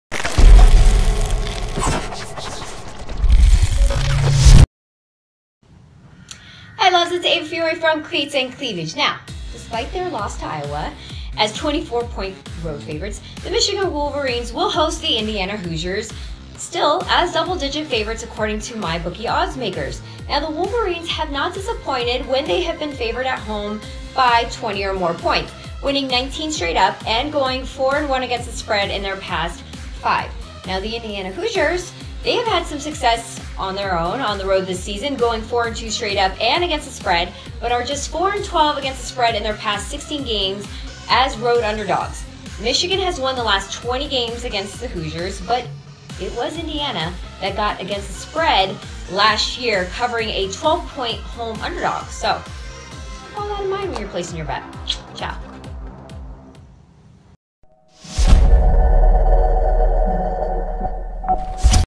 Week 12 College Football Betting Odds Indiana vs Michigan
Despite their loss to Iowa as 24-point road favorite, the Michigan Wolverines will host the Indiana Hoosiers as double digit favorite according to Mybookie Oddmakers.

The Wolverines have not disappointed when favored at home by 20 or more points, winning 19 straight SU, and going 4-1 ATS in their past five. The Indiana Hoosiers had some success on the road this season, going 4-2 SU and ATS, but are just 4-12 ATS in their past 16 games as road underdogs.

Michigan has won the last 20 games against the Hoosiers, but it was Indiana that got the ATS win last year, covering as 12-point home underdogs.